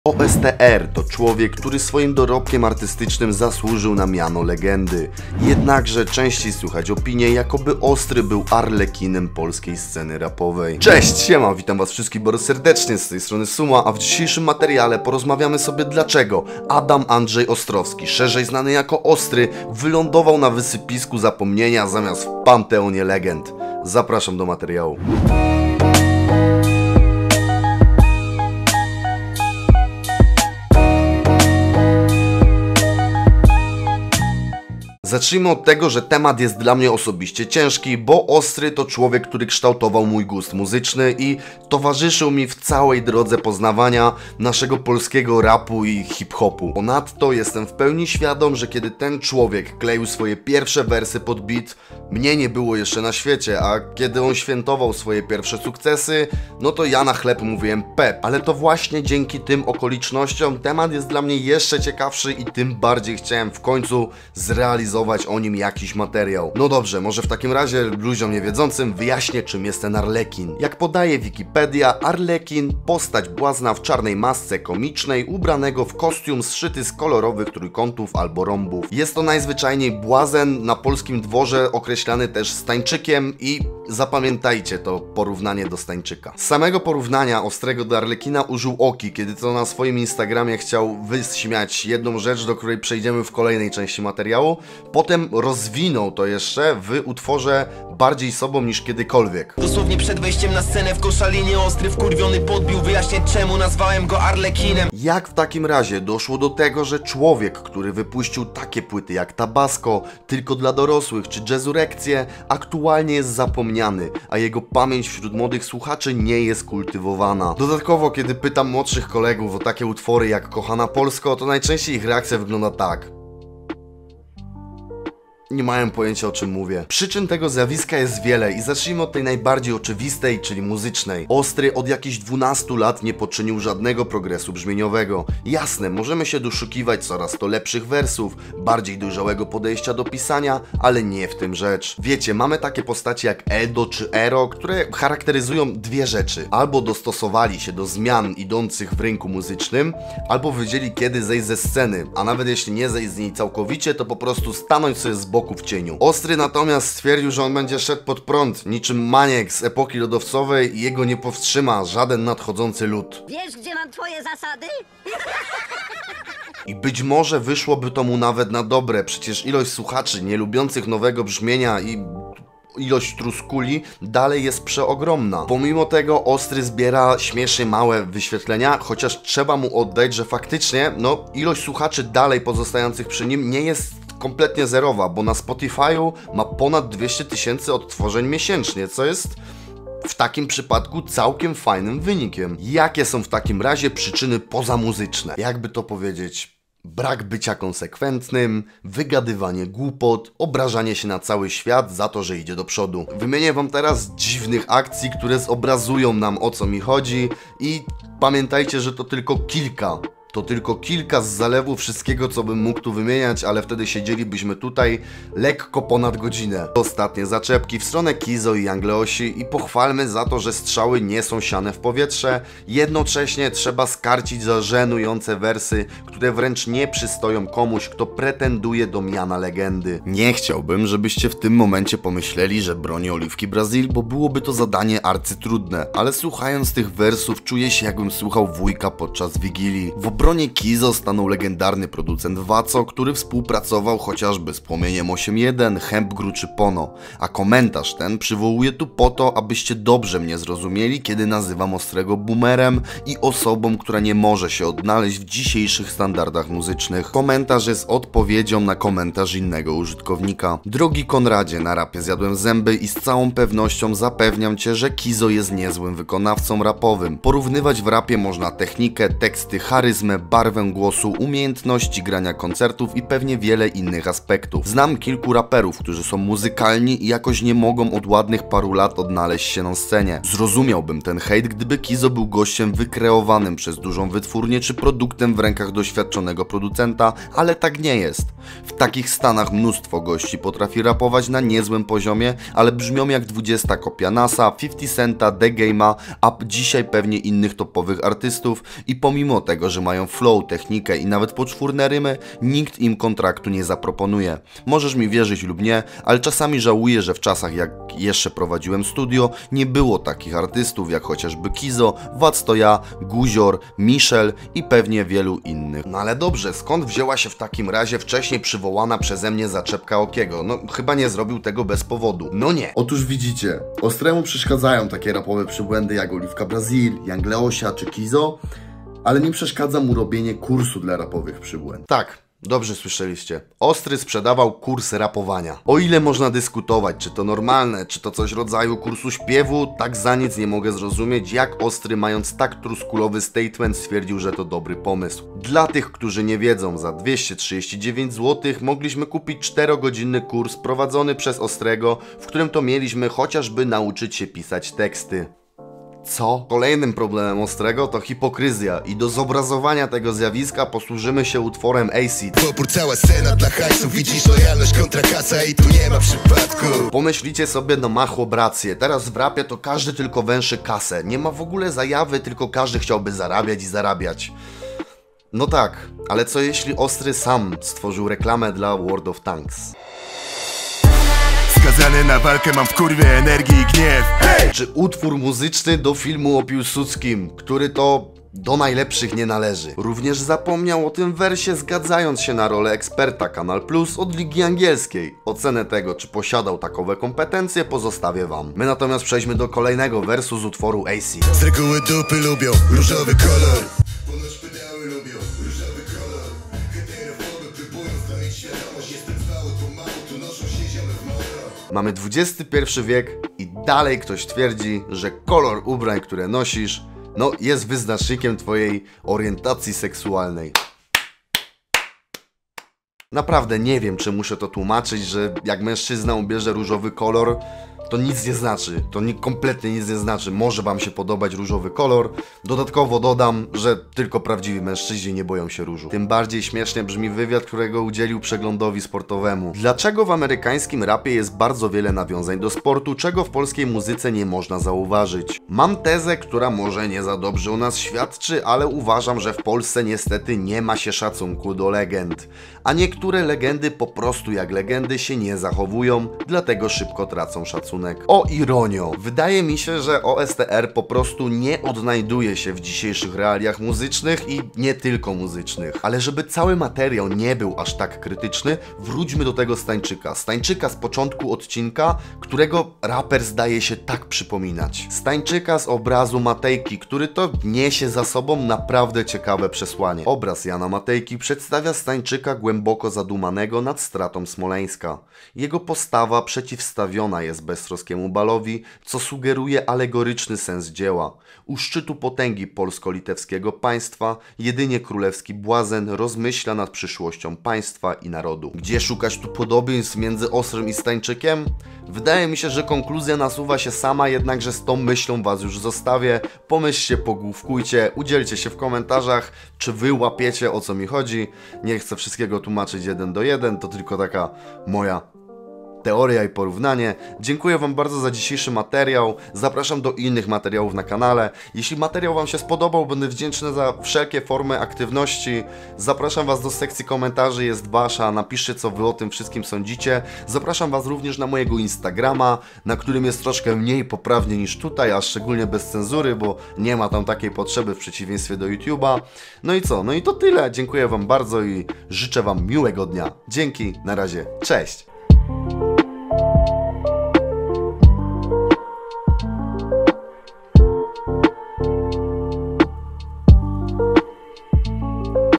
O.S.T.R od LEGENDY do ARLEKINA!
OSTR to człowiek, który swoim dorobkiem artystycznym zasłużył na miano legendy. Jednakże częściej słychać opinie, jakoby OSTRY był arlekinem polskiej sceny rapowej. Cześć, siema, witam Was wszystkich bardzo serdecznie, z tej strony Suma, a w dzisiejszym filmie porozmawiamy sobie dlaczego Adam Andrzej Ostrowski, szerzej znany jako OSTR wylądował na wysypisku zapomnienia, zamiast w panteonie Legend. Zapraszam do materiału!